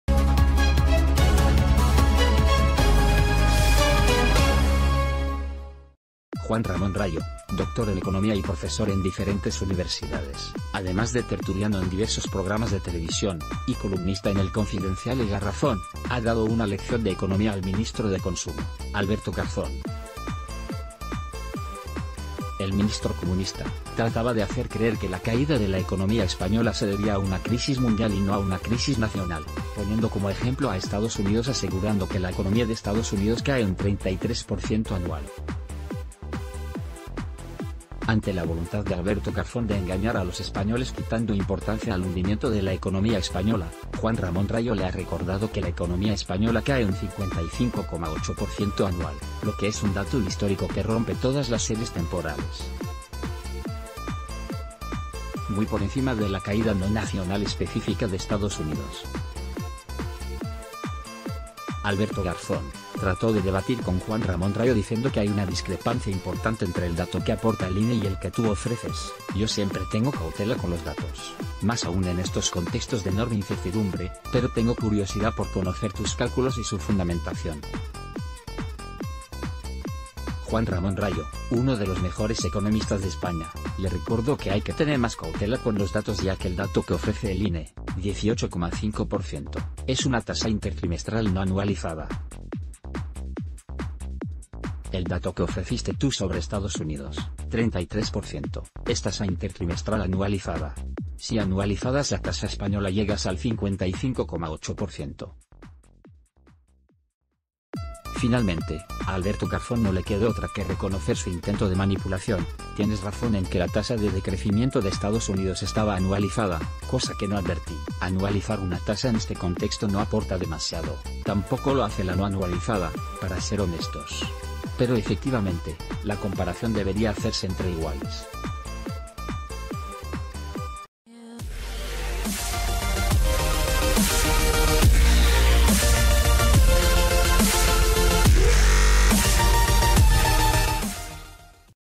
Juan Ramón Rallo hu.milla a Alberto Garzón en Twitter con una lección de economía: «Hay que tener...
Juan Ramón Rallo hu.milla a Alberto Garzón en Twitter con una lección de economía: «Hay que tener cautela con los datos»